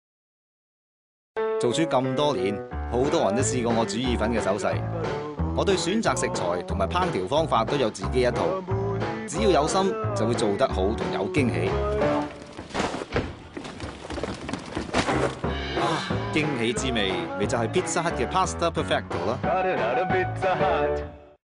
Pizza Hut - Pasta Perfetto Commercial TVC 電視廣告 2011 Road Show
Actors, 演員:
AL LEUNG
Hallelujah Hale Konno
Rainbow Yeung
Ricky Cheung